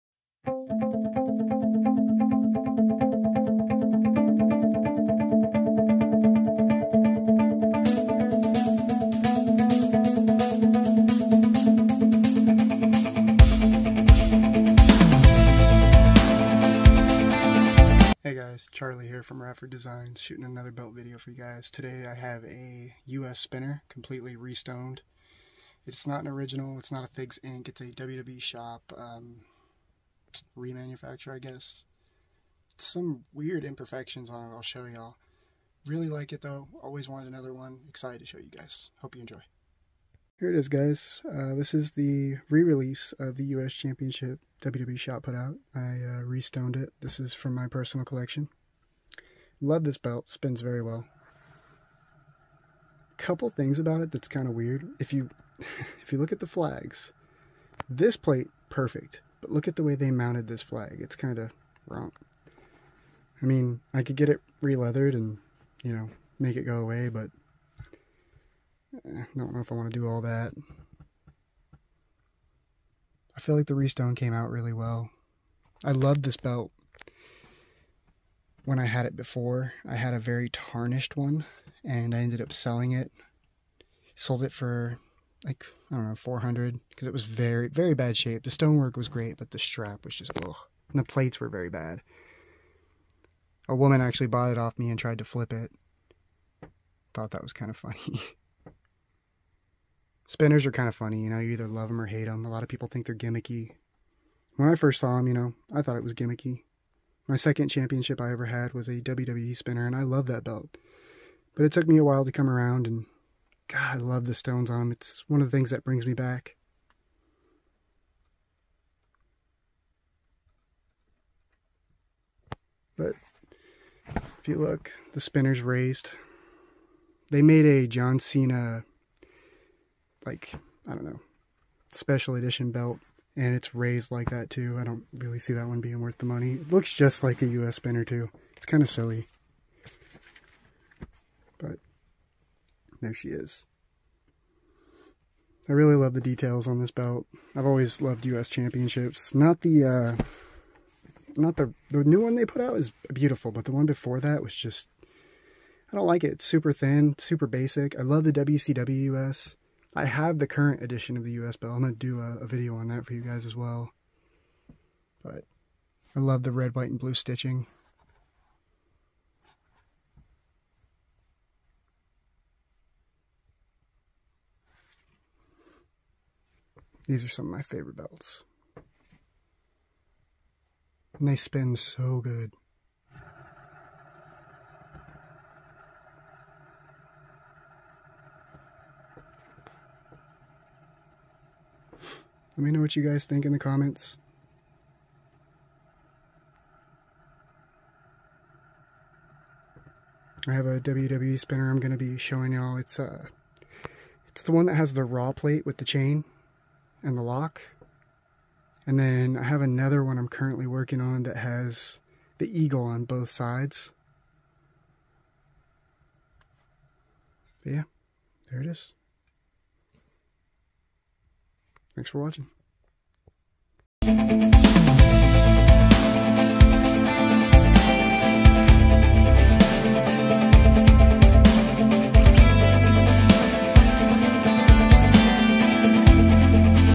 WWE U.S. Spinner Replica re-stoned
WWE U.S. championship replica re-stoned by Rafford designs